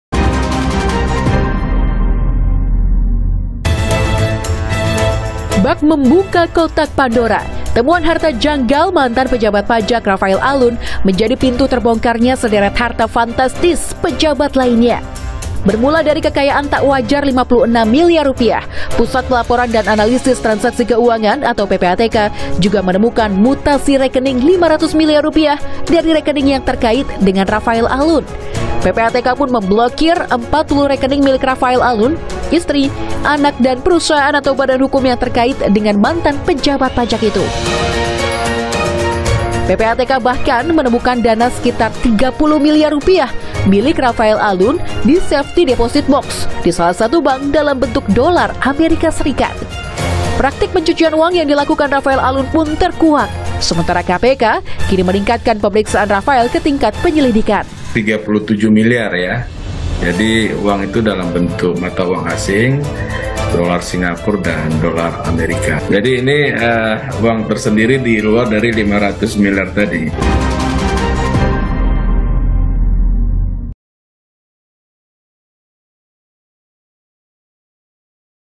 PPATK Blokir 40 Rekening Rafael Alun, Mutasinya hingga 500 Miliar Rupiah | Laporan Khusus
KOMPASTV - Harta janggal yang dimiliki Rafael Alun Trisambodo kini memasuki babak baru. Mantan pejabat Direktorat Jenderal Pajak Kemenkeu itu diungkap PPATK (Pusat Pelaporan dan Analisis Transaksi Keuangan) ada transaksi janggal senilai Rp 500 miliar.

Ada sebanyak 40 rekening pribadi milik Rafael Alun Trisambodo dan keluarga yang diblokir atau dibekukan. Hal itu dibenarkan Pusat Pelaporan dan Analisis Transaksi Keuangan (PPATK) saat dikonfirmasi pada Selasa, 7 Maret 2023.

"Ada lebih dari 40 rekening sudah kami blokir," ujar Kepala PPATK Ivan Yustiavandana kepada Kompas.com, Selasa (7/3/2023). Ia menuturkan, pemblokiran rekening tersebut mencakup rekening milik Rafael Alun Trisambodo, keluarga, serta pihak lain yang terkait.

Adapun nilai transaksi dari puluhan rekening tersebut mencapai Rp 500 miliar selama periode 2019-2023. "Nilai mutasi rekeningnya dalam periode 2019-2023 sekitar Rp 500 miliar," ucapnya.